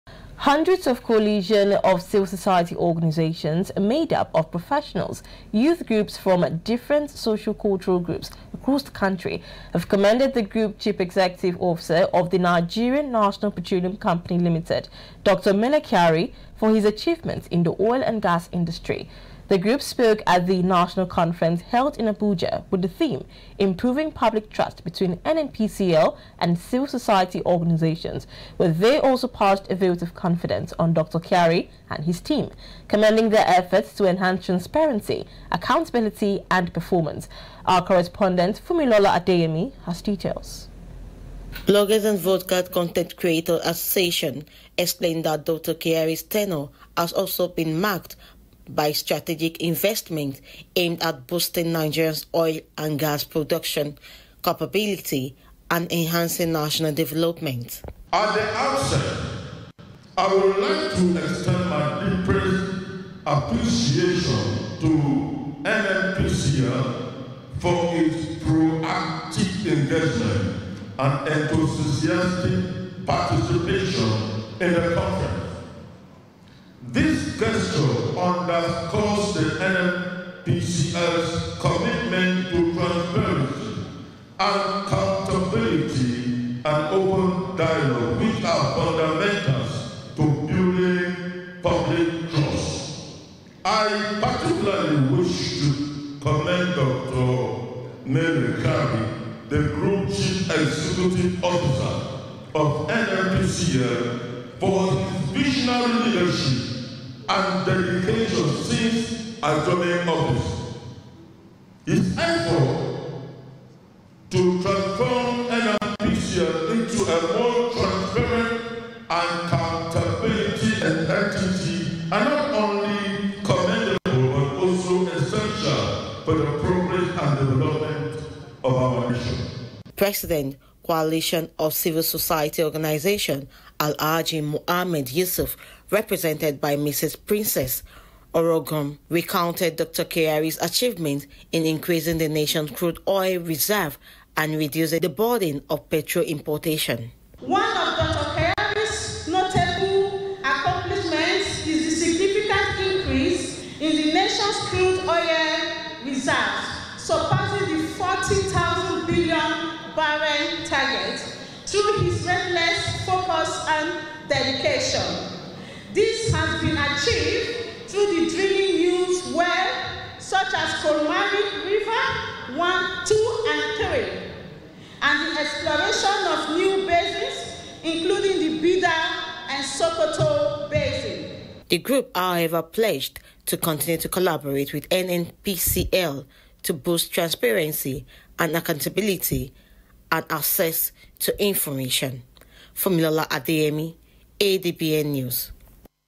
NNPCL: COALTION OF CIVIL SOCIETY ORGANIZATIONS PASSES VOTE OF CONFIDENCE ON MELE KYARI
Hundreds of Coalition of Civil Society Organisations, made up of professionals, youth groups from different social cultural groups across the country have commended the Group Chief Executive Officer of the Nigerian National Petroleum Company Limited, Dr. Mele Kyari, for his  achievements in the oil and gas industry.